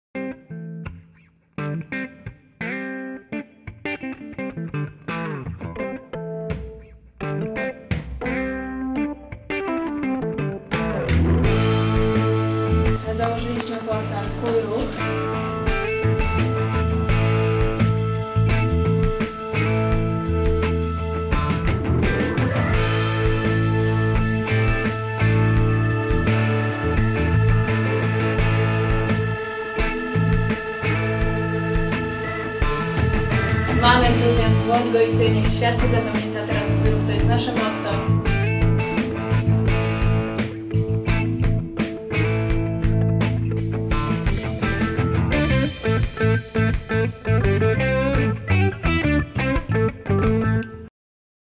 Pecha Kucha Lublin - trailer
Pecha Kucha [peczakcza], czyli spotkanie ludzi, którzy chcą zaprezentować swoje pasje, inicjatywy. Ciekawa w formie, ciekawa w treści. Twój Ruch również miał okazję się tam przedstawić. Zapraszamy na krótki trailer, a już za jakiś czas relacja z całości spotkania.